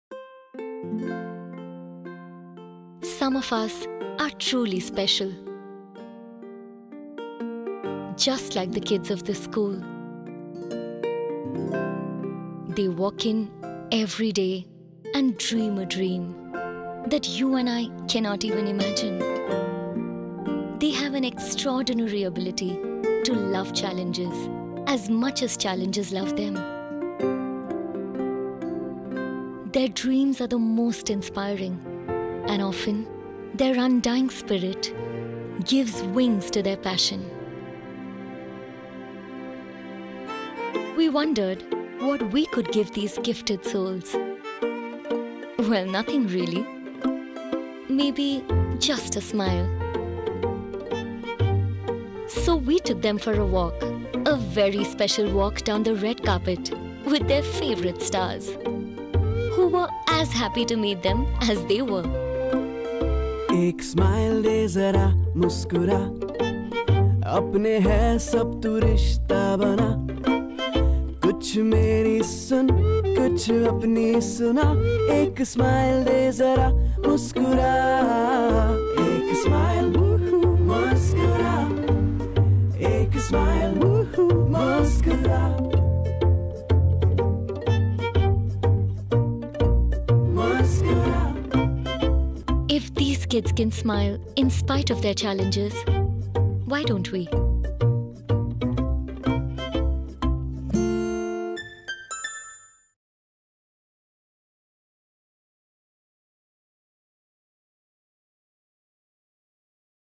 Good Day- Britannia Filmfare Awards South 2016
The time when special kids gave the Indian film stars a special gift- a lasting smile on their faces